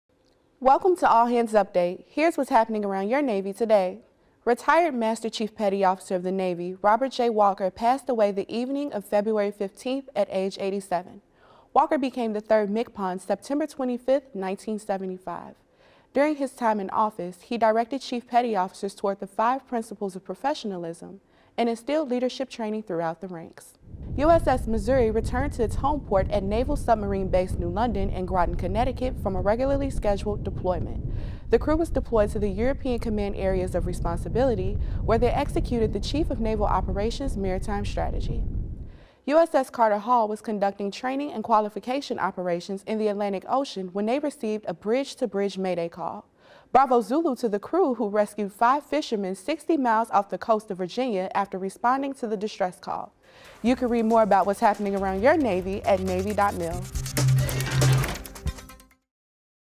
Headlines for Thursday, February 17, 2016 (HL-17)
Navy's 3rd MCPON Passes. USS Missouri Returns from 6-Month Deployment. USS Carter Hall Rescues Fishermen in Distress.